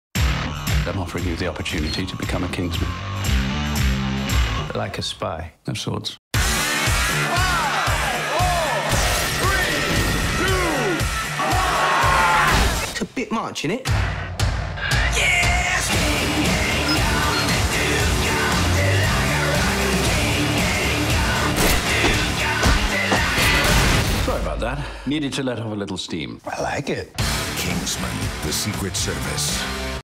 Kingsman: The Secret Service | Camelot Reviews
Kingsman: The Secret Service tells the story of a super-secret spy organization that recruits an unrefined but promising street kid into the agency's ultra-competitive training program just as a global threat emerges from a twisted tech genius.

Written by Jane Goldman & Matthew Vaughn, based upon the comic book by Mark Millar & Dave Gibbons

Cast: Colin Firth, Michael Caine, Taron Egerton, and Samuel L. Jackson